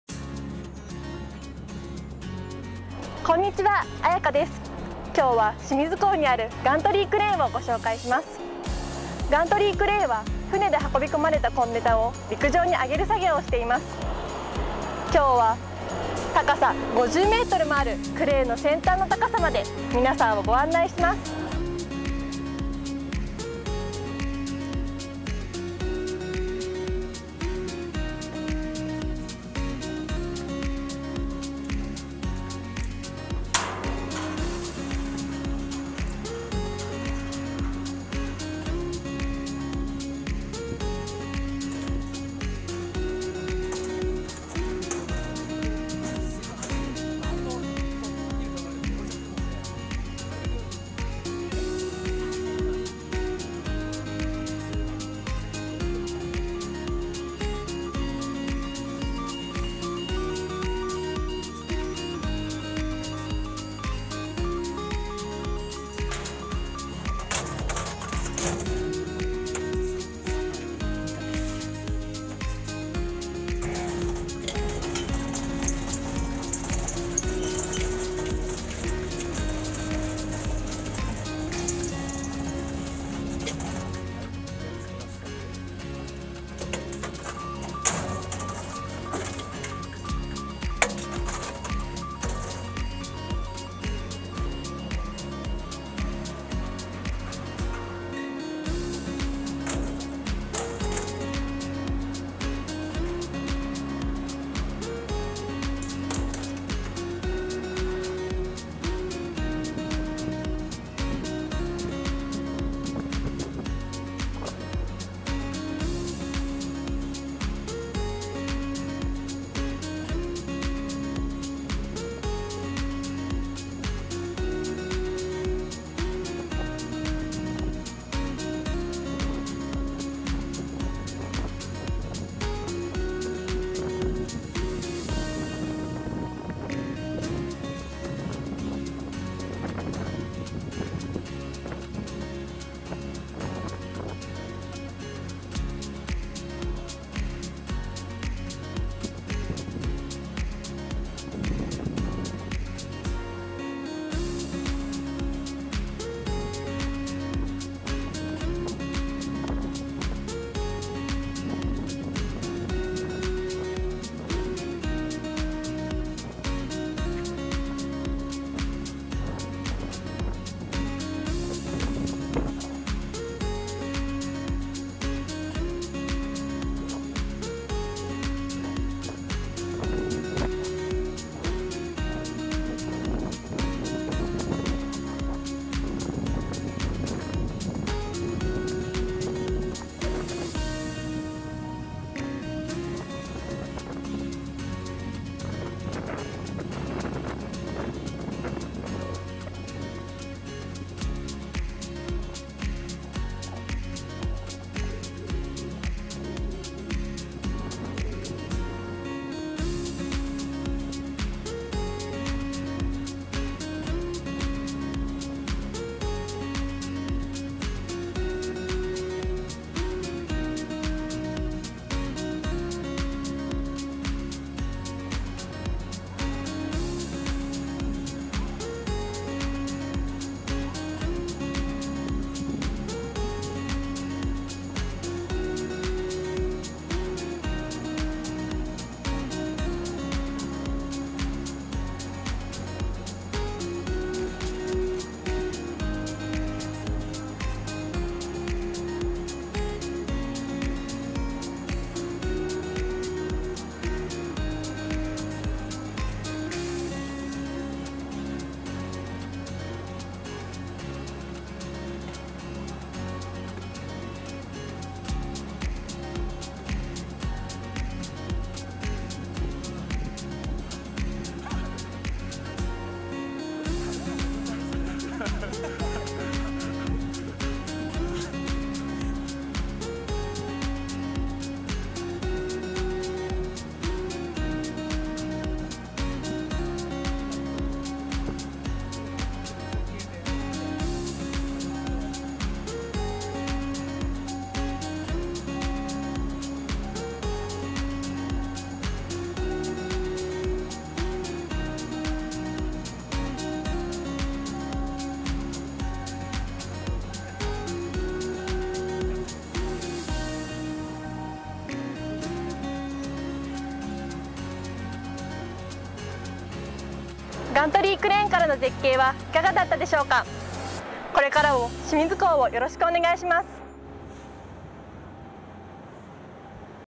清水港 ガントリークレーン VR 新興津コンテナクレーン３号機
清水港のガントリークレーンをVR映像でご覧いただけます。高さ50メートルの清水港ガントリークレーン3号機からの体験ができます。撮影日は晴天のため、富士山も綺麗に見ることができます。

2020年2月に設置したガントリークレーン6号機のVRは、こちらから

Enjoy the thrill and  the beauty from the top of the gantry crane  at the top of the Gantry Crane at the Port of Shimizu with this VR.